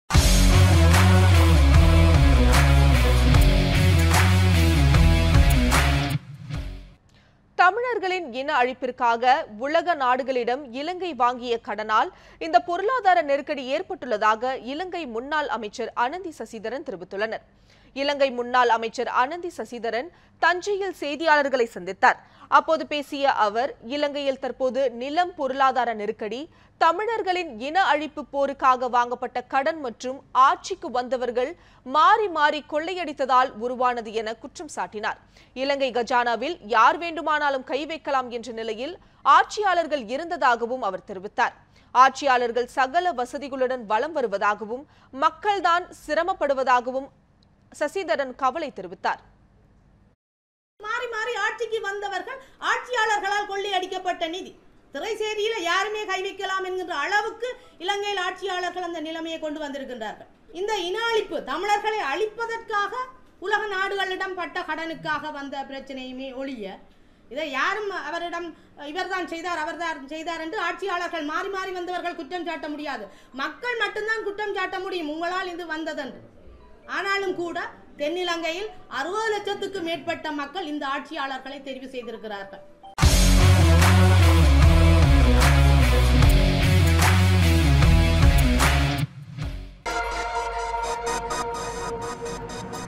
உலக நாடுகளிடம் இலங்கை வாங்கிய கடனால் இந்த பொருளாதார நெருக்கடி ஏற்பட்டுள்ளது
உலக நாடுகளிடம் இலங்கை வாங்கிய கடனால் இந்த  பொருளாதார நெருக்கடி ஏற்பட்டுள்ளது - இலங்கை முன்னாள் அமைச்சர்  அனந்தி சசிதரன்